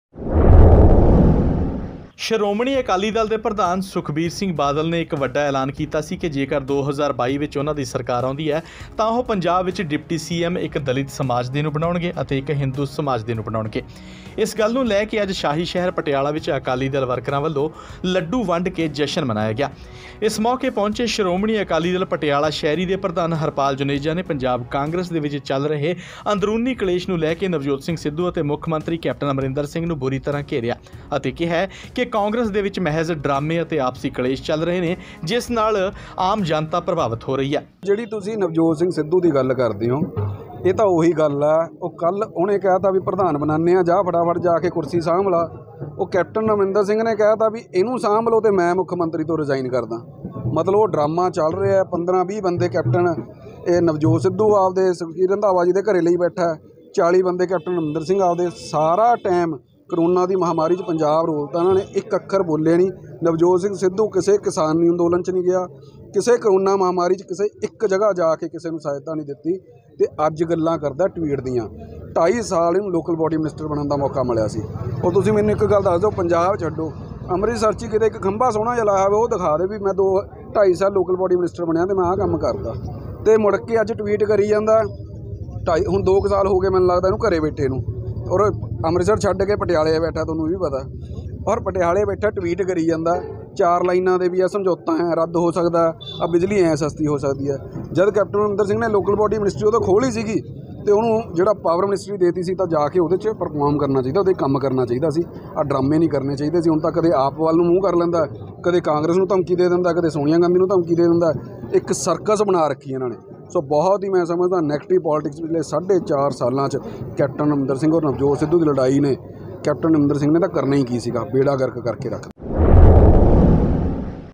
Punjab Congress 'ਚ ਚੱਲ ਰਹੀ ਹੈ ਸਰਕਸ, ਜਨਤਾ ਦਾ ਹੋ ਰਿਹਾ ਨੁਕਸਾਨ: ਅਕਾਲੀ ਆਗੂ
ਪੰਜਾਬ ਕਾਂਗਰਸ ਦੇ ਵਿਚ ਚੱਲ ਰਹੇ ਅੰਦਰੂਨੀ ਕਲੇਸ਼ ਨੂੰ ਲੈਕੇ ਹੁਣ ਸ਼ਿਰੋਮਣੀ ਅਕਾਲੀ ਦਲ ਨੇ ਨਵਜੋਤ ਸਿੰਘ ਸਿੱਧੂ ਅਤੇ ਮੁਖ ਮੰਤਰੀ ਕੈਪਟਨ ਅਮਰਿੰਦਰ ਸਿੰਘ ਨੂੰ ਬੁਰੀ ਤਰ੍ਹਾਂ ਨਾਲ ਘੇਰਿਆ ,,,,, ਪਟਿਆਲਾ ਸ਼ਹਿਰੀ ਦੇ ਅਕਾਲੀ ਪ੍ਰਧਾਨ ਹਰਪਾਲ ਜੁਨੇਜਾ ਨੇ ਕਿਹਾ ਕਿ ਕੈਪਟਨ ਅਤੇ ਸਿੱਧੂ ਦੀ ਲੜਾਈ ਵਿਚਕਾਰ ਪੰਜਾਬ ਦੇ ਲੋਕਾਂ ਦਾ ਬੁਰਾ ਹਾਲ ਹੋ ਰਿਹਾ ਹੈ ,, ਹੋਰ ਕੀ ਕਿਹਾ ਹਰਪਾਲ ਜੁਨੇਜਾ ਨੇ ਸੁਣਦੇ ਆਂ